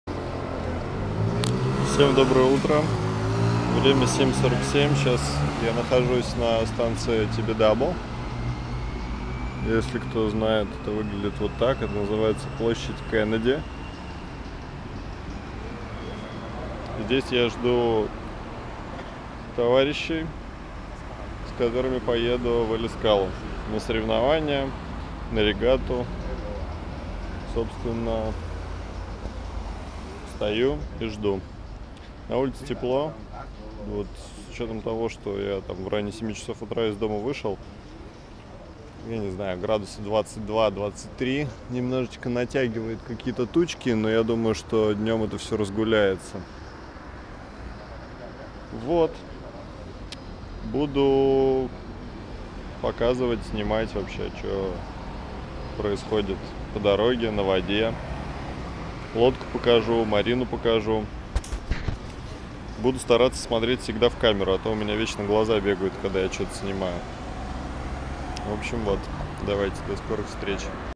утро в Барселоне, перед регатой
Утро, жду когда меня заберут и мы поедем в Эль Эскалу на регату